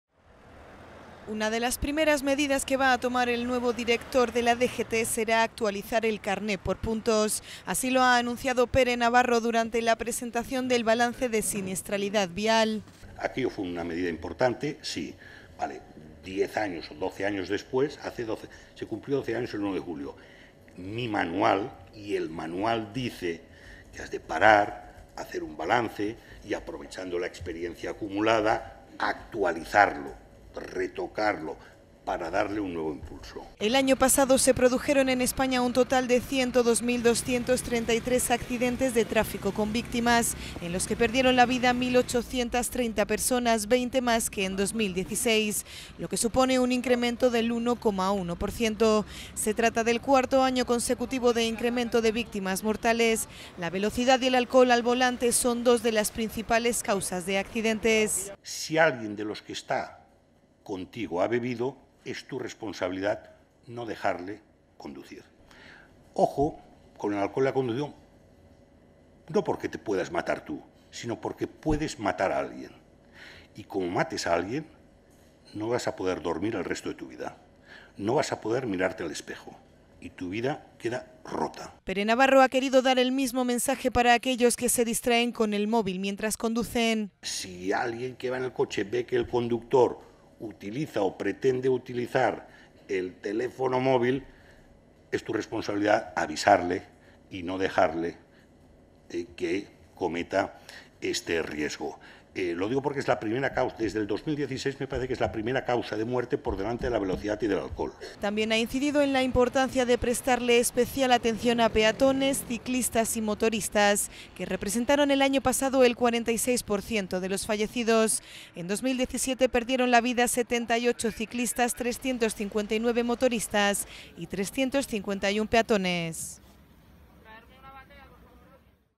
DGT sobre carnet por puntos: "Hay que hacer balance, actualizar y retocar"
El director general de Tráfico, Pere Navarro, ha anunciado en su primera comparecencia pública tras tomar posesión que una de sus primeras medidas al frente del departamento será la actualización del carné por puntos. "Hay que hacer balance, actualizar y retocar para darle un nuevo impulso", ha precisado.